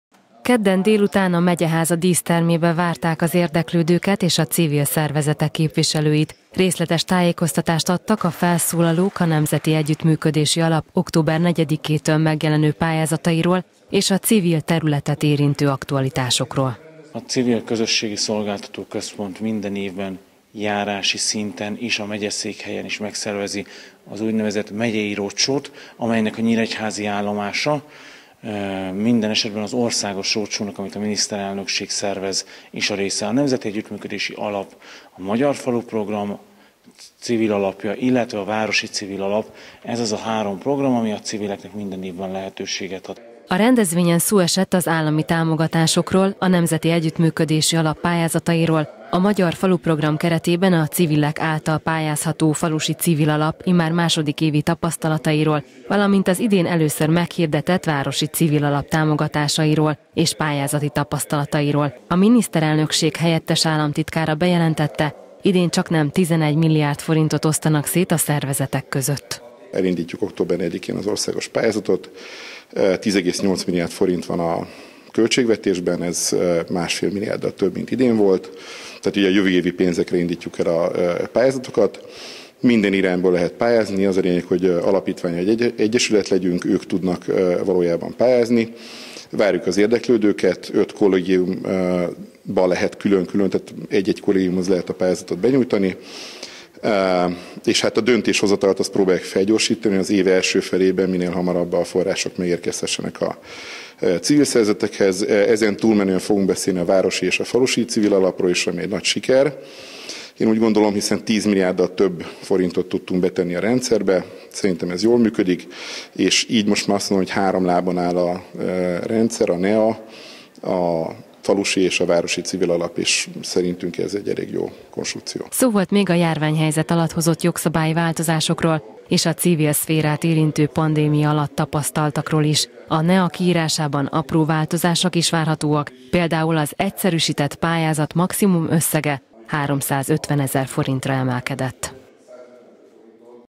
Pályázatok civileknek
Október negyedikétől lehet pályázni a Nemzeti Együttműködési Alap forrásaira civil szervezeteknek.
A feltételekről és a jogszabályi változásokról tartottak fórumot kedden délután Nyíregyházán. Szalay-Bobrovniczky Vince, a Miniszterelnökség helyettes államtitkára bejelentette: a mostani kiírásban 10,8 milliárd forintra pályázhatnak a szervezetek.